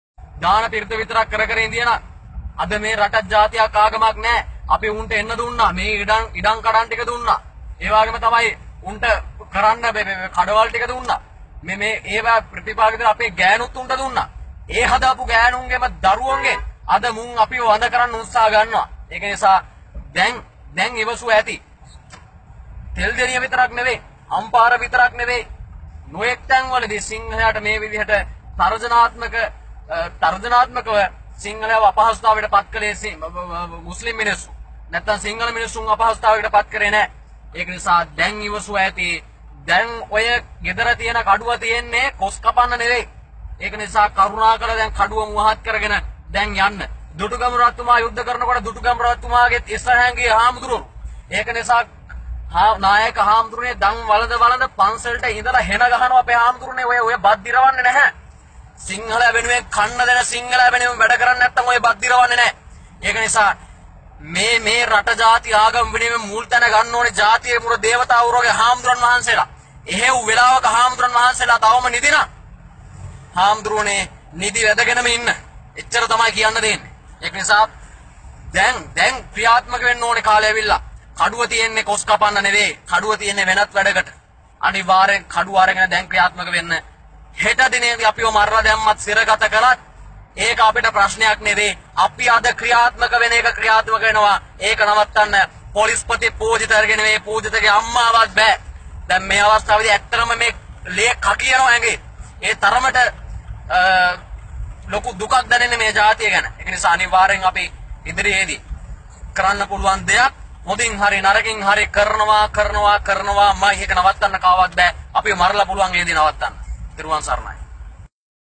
Buddhist Monk Who Perpetrated Anti-Muslim Violence
'Whether Pujitha (IGP Pujitha Jayasundera) comes or his mother comes, these monks will not be stopped, " he said. While traveling to Digana he urged people to sharpen their swords and come forward the day anti-muslim attacks start.